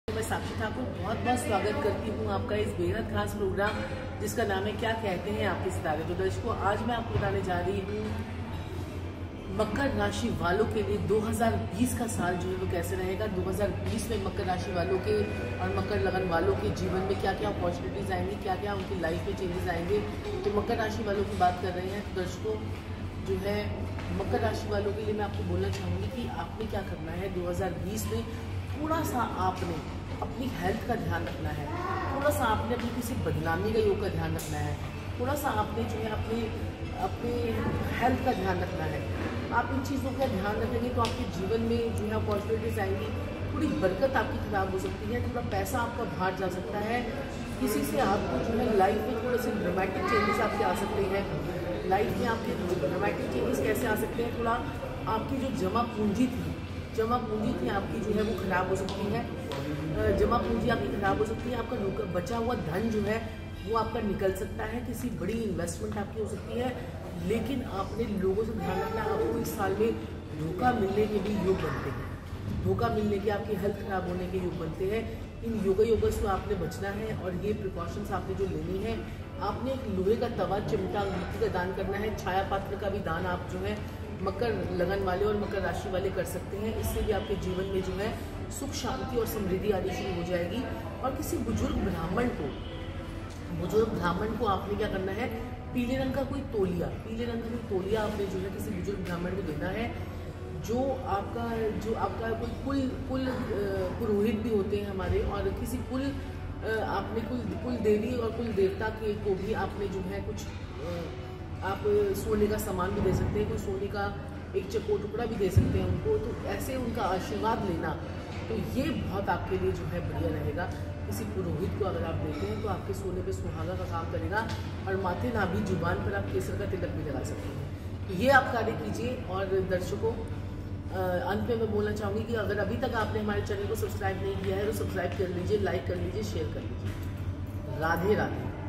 मकर राशि / लग्न वालों को क्या मिलेगा 2020 में ? | क्या करें प्रयोग । कैसे हो पूरा साल बेहतरीन ?
5 जून चंद्र ग्रहण स्पेशल कपूर के चमत्कारी और अध्भुत उपाय जो तुरंत असर डाले मिटाए आपकी सभी समस्यायें
world best astrologer, celebrity astrologer, vastu expert

Kya kehte hai apke sitare..
Prediction from Best Astrologer in Delhi
Acharya Sakshi Sanjeev Thakur..